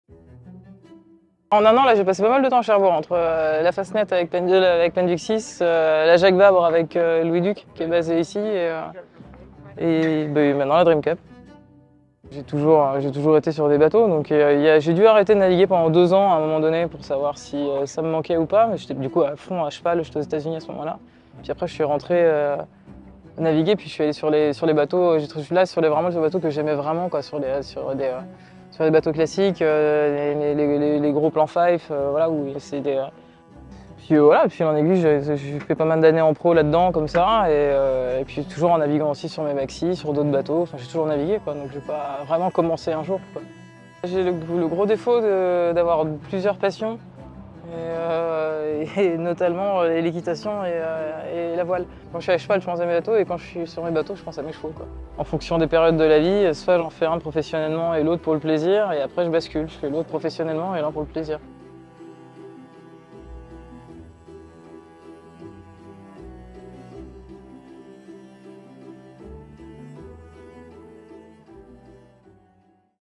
Paroles de skipper : Marie Tabarly
📣 ParolesDeSkipper ⚓Faire de la voile, une évidence pour Marie Tabarly qui a baigné dedans depuis son enfance.
⛵ Elle sera une des trop rares femmes à prendre le départ de la course, à bord de son Pen Duick 6.
La Ville l’a invitée au coté de plusieurs skippeuses, pour participer à une table ronde dédiée aux Femmes dans la voile.
👉 Rendez-vous le samedi 16 juillet à 15h sur le village de la course, plage verte.